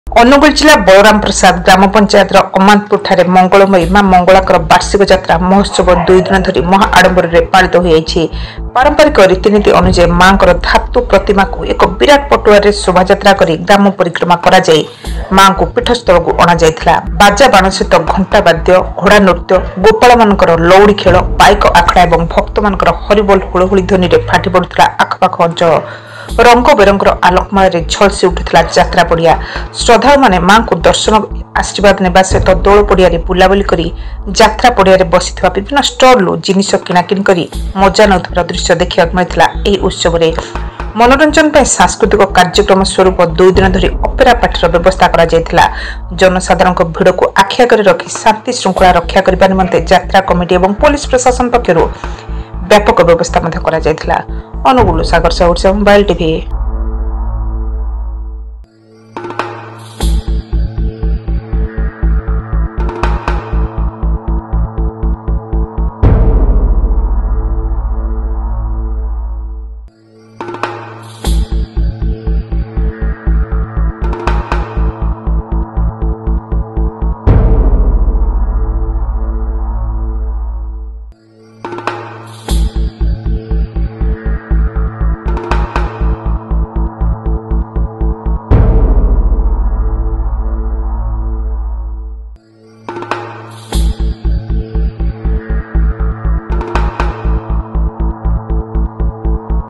ଅନୁଗୁଳରେ ମା ମଙ୍ଗଳାଙ୍କ ବାର୍ଷିକ ଯାତ୍ରାରେ ଉତ୍ସାହ#odishamobiletv
ସାରା ଦୁନିଆଁର ଟିକ୍ନିକ୍ ଖବରକୁ ନେଇ ଭିନ୍ନଢଙ୍ଗରେ ପହଞ୍ଚାଇବାର ପ୍ରୟାସ,ଅନୁଗୁଳରେ ମା ମଙ୍ଗଳାଙ୍କ ବାର୍ଷିକ ଯାତ୍ରାରେ ଉତ୍ସାହ#odishamobiletv